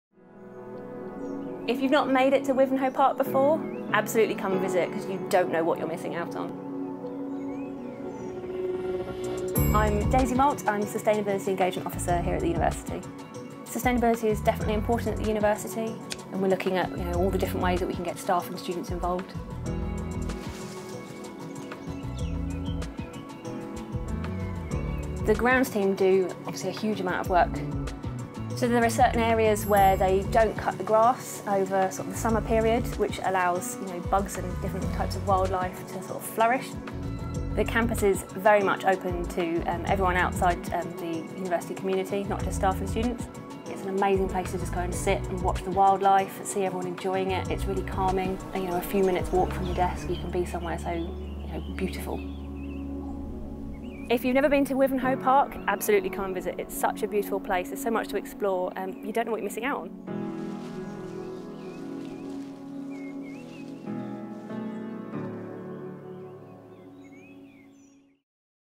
The People’s Choice Awards – sustainability and biodiversity, University of Essex
The University of Essex has set ambitious targets for reducing carbon emissions and encouraging sustainability on our campuses. Daisy Malt, Sustainability Engagement Officer, discusses Wivenhoe Park and the steps our Grounds Team are taking to encourage biodiversity.